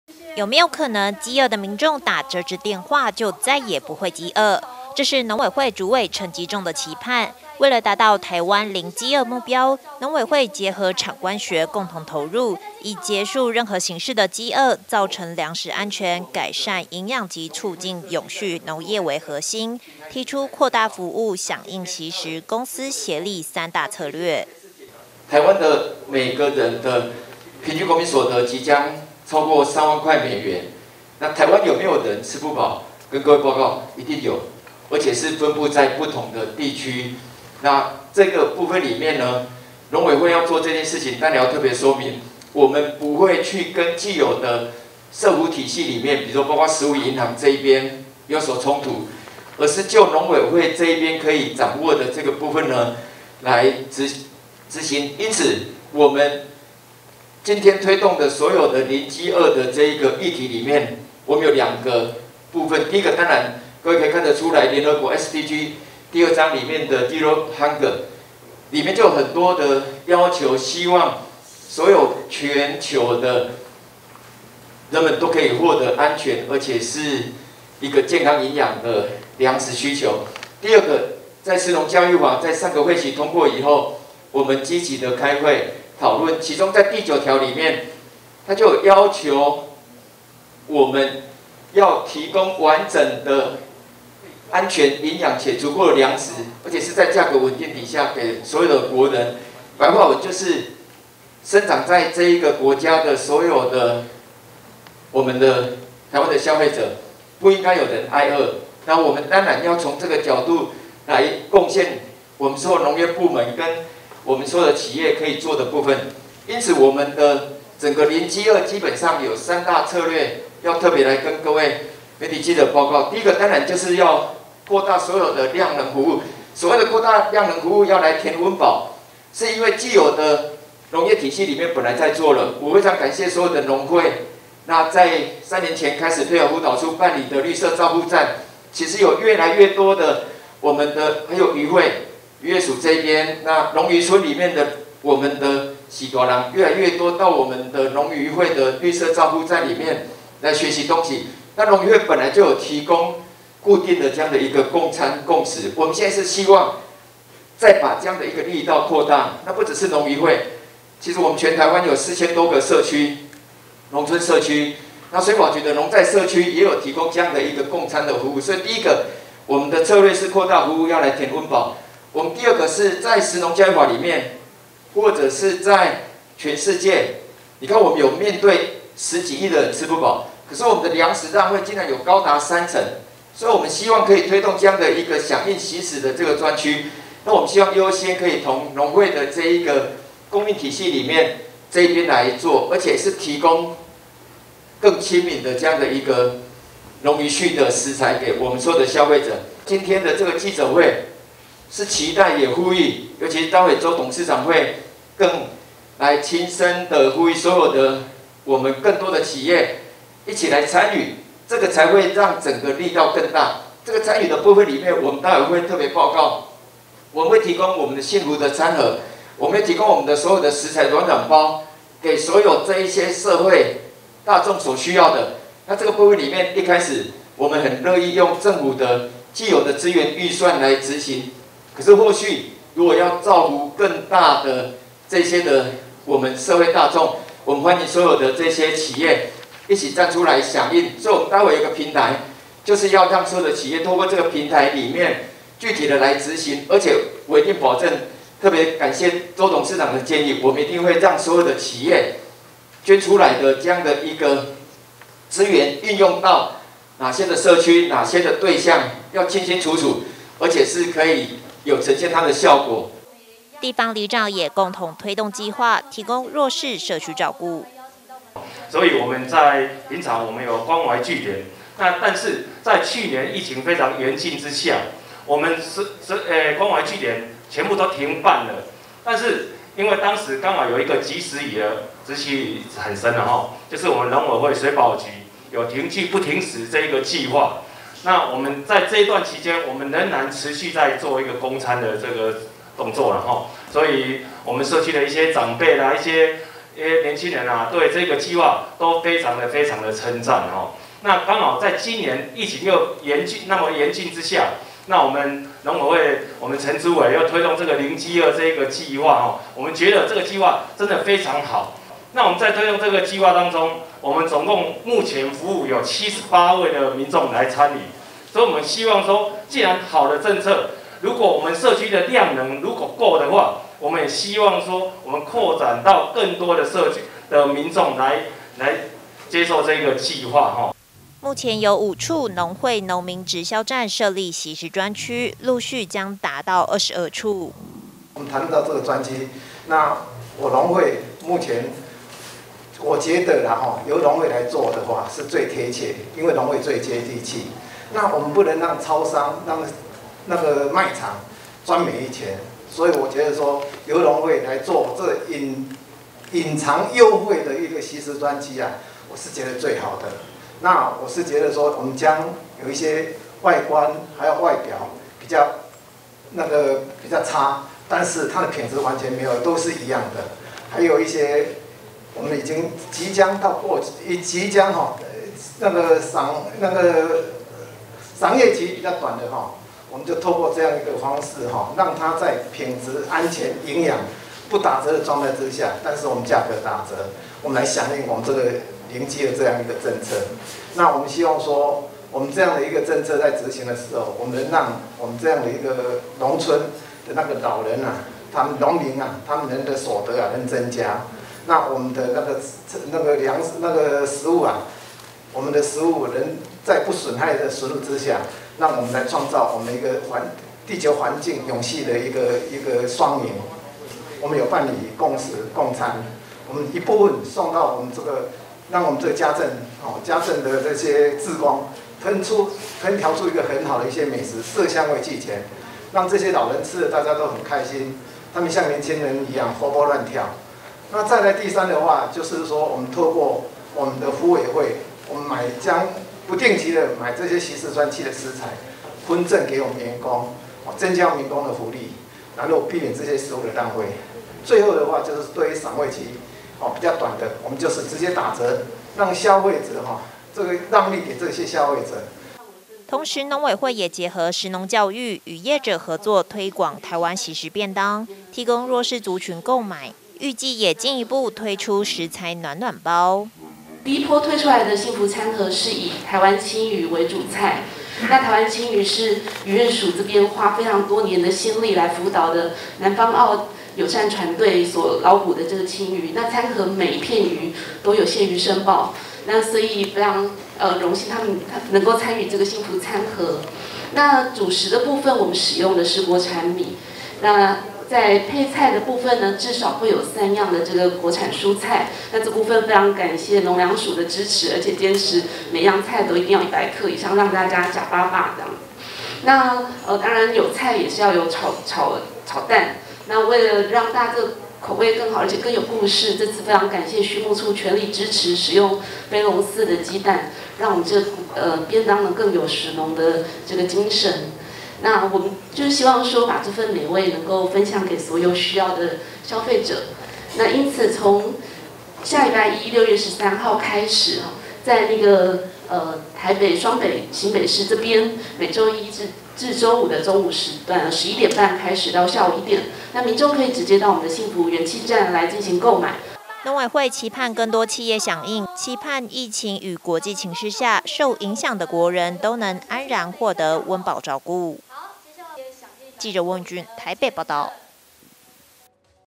惜食據點|農委會攜手產官學界 達台灣零飢餓目標
農委會攜手產官學界 達台灣零飢餓目標

要實現全台灣零飢餓的目標，農委會攜手企業、學校、基金會，透過拓展服務據點、響應惜食、公私協力等策略，實現目標。